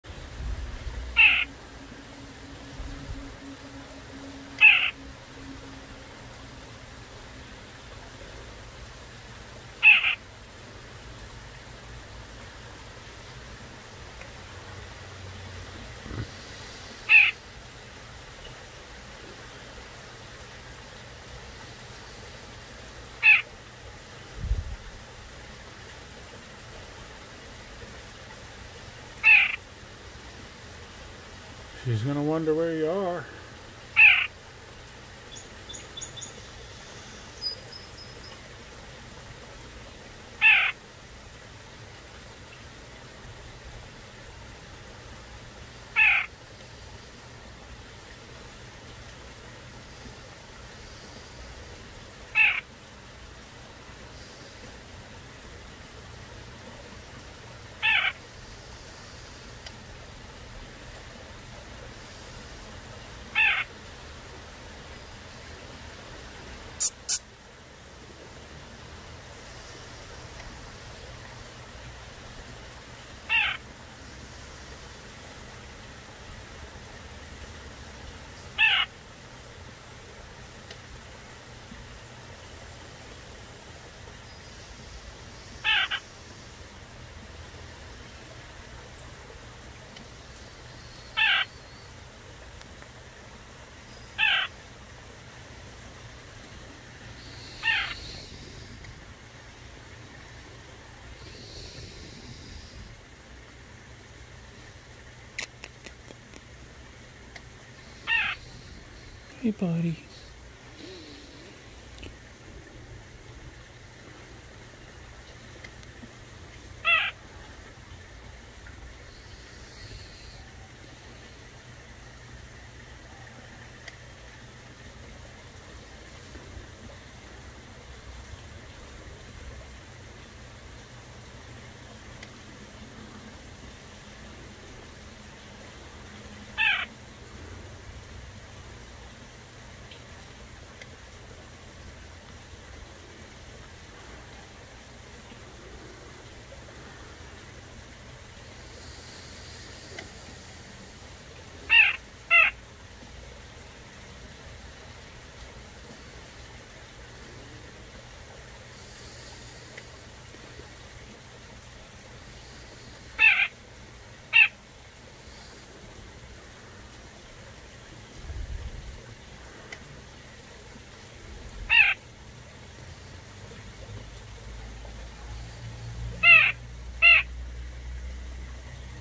Befriending a Nuthatch baby bird
Befriending a Nuthatch baby bird short version.